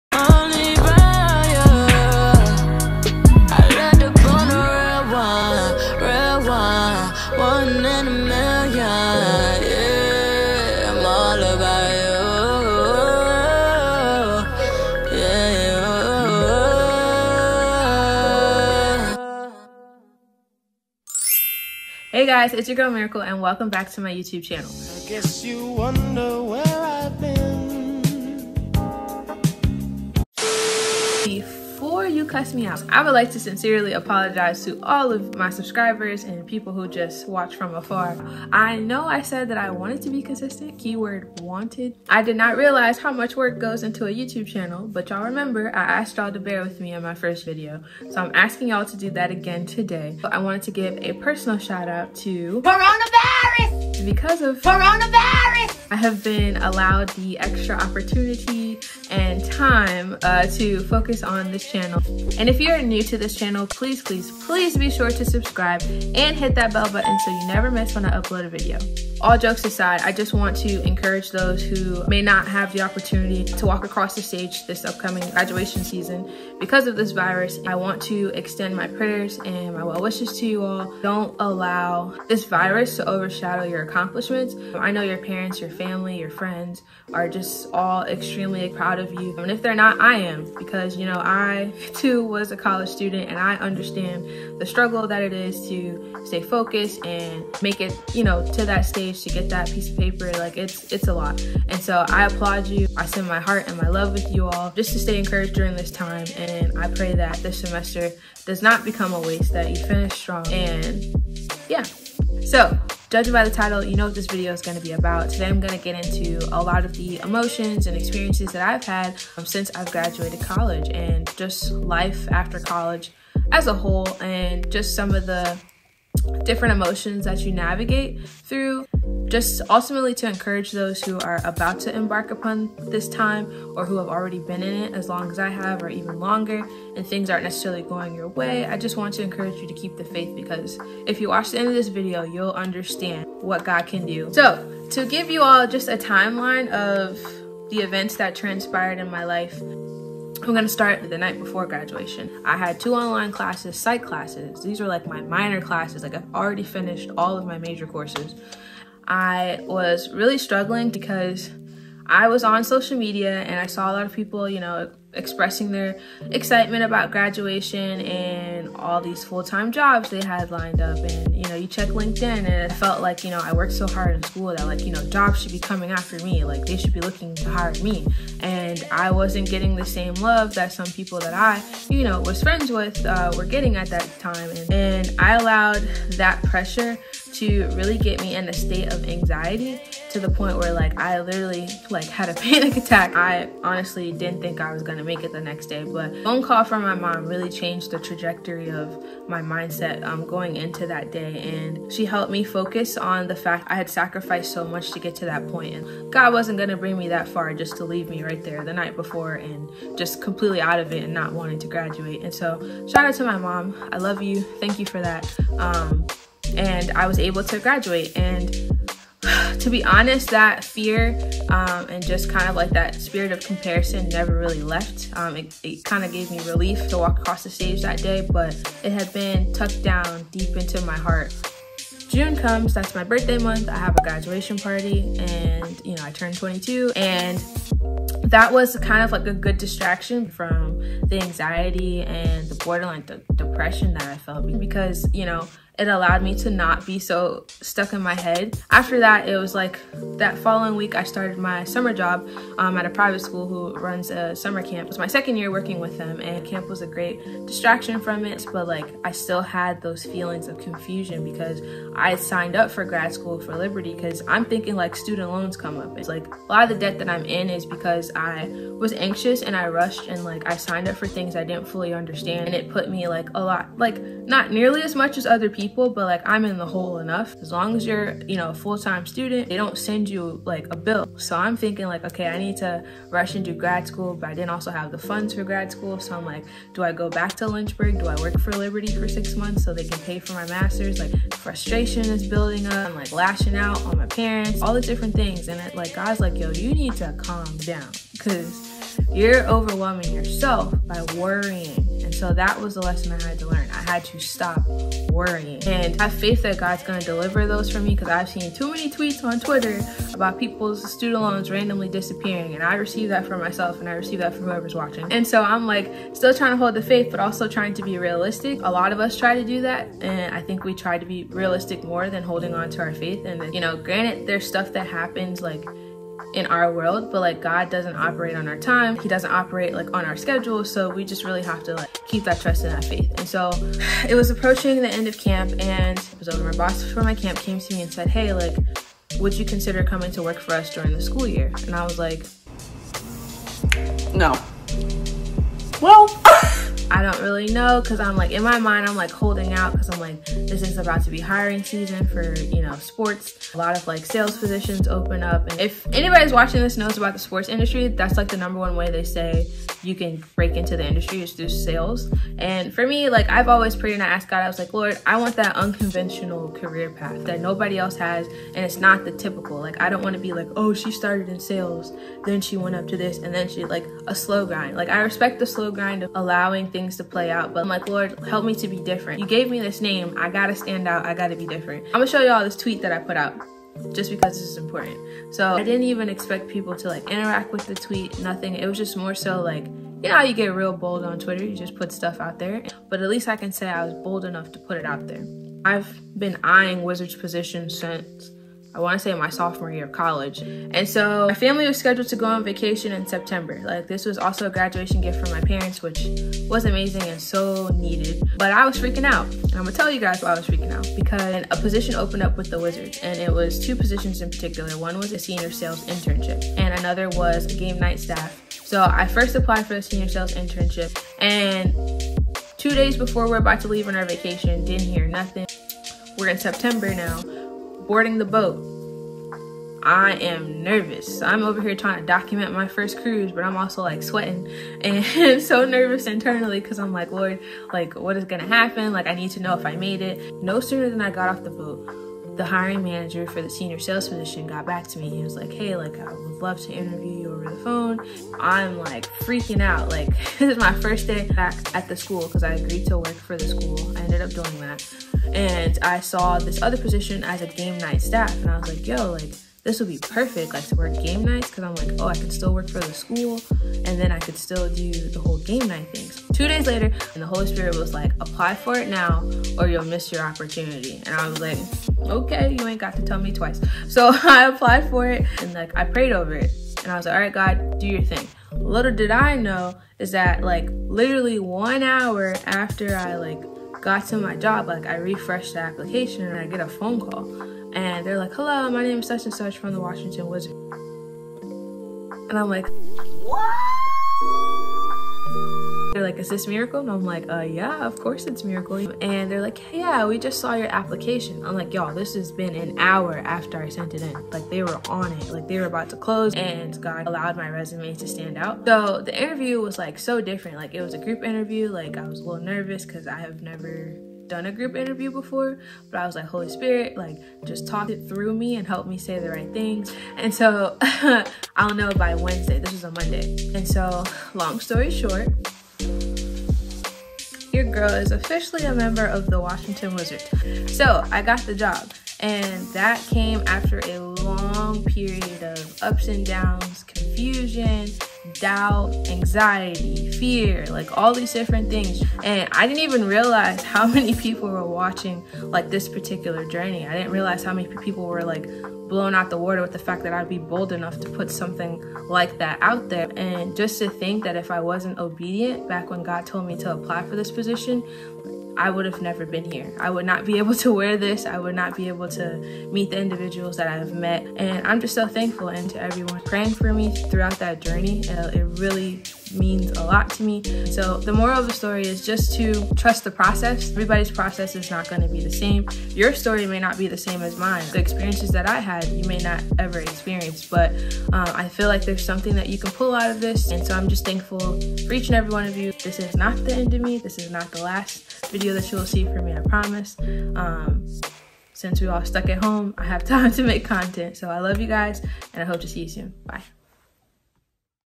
LIFE AFTER COLLEGE | Liberty University | Miracle Pearsall
I'M BACK!

Broke my silence during this pandemic to give you all an update on what's been happening in my life since I graduated from Liberty University last May. I talk about anxiety, depression, jobs, student loans and more, with a special message to the Class of 2020! Hope you like it...